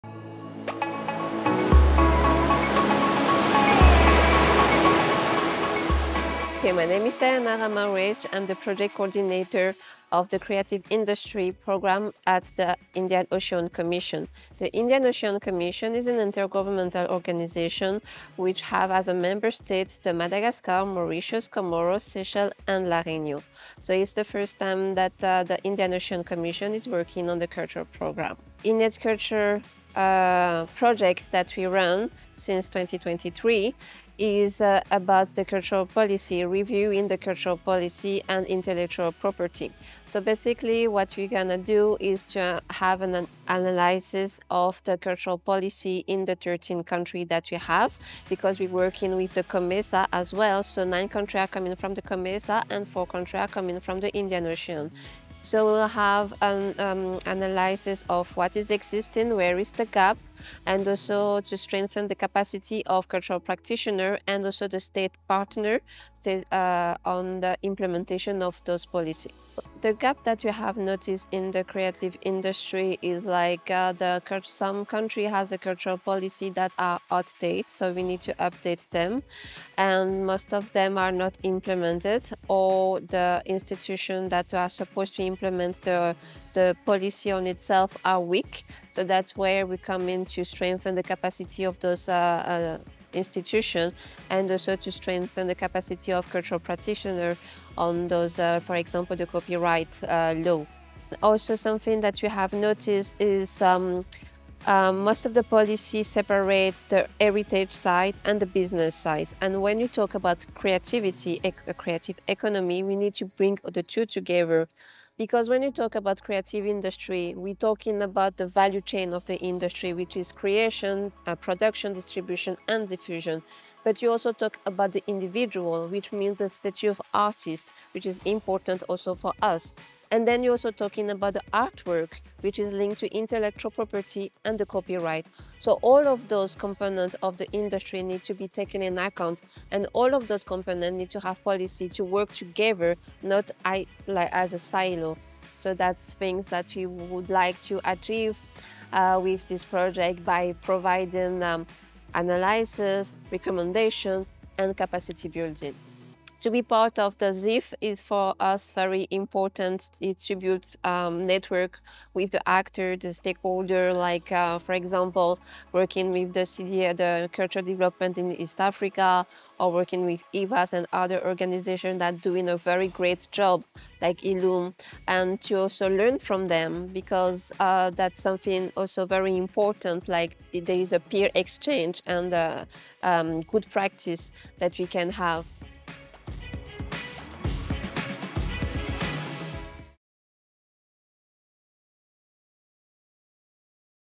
1 on 1 with Diana Ramarohetra, Project Coordinator at Indian Ocean Commission (IOC)
The IOC is embarking on an in depth analysis of cultural policy frameworks in Ethiopia, Burundi, Comoros Islands, Seychelles, Kenya, Mauritius, Rwanda, Uganda, Madagascar and Djibouti as part of their granted project. They are working towards providing a better understanding and marrying the heritage side of CCIs with the business side as well, by putting together policies that will allow both factors to work together int he creative economy space.